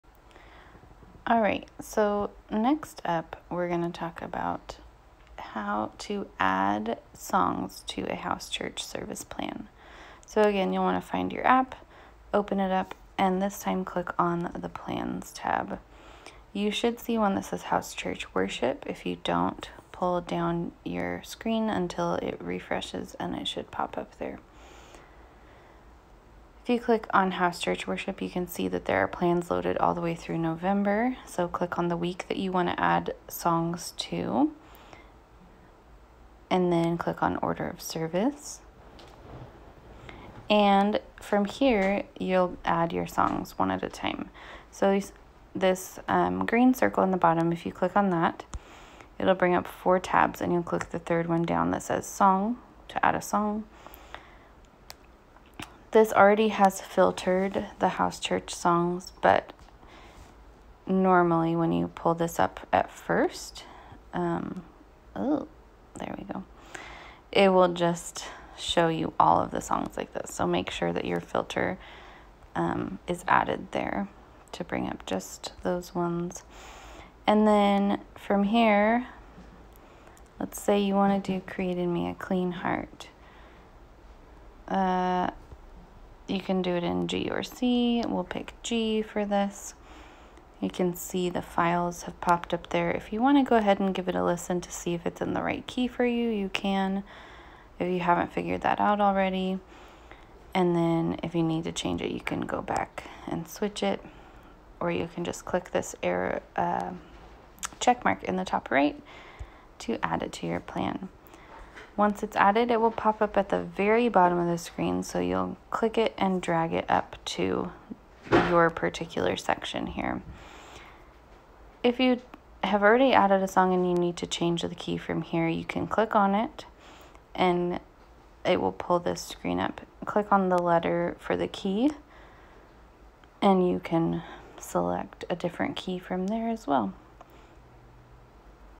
How to add House Church songs to a plan on PCO (mobile)
This video will teach you how to add songs to a House Church service plan on Planning Center Services while using an iPhone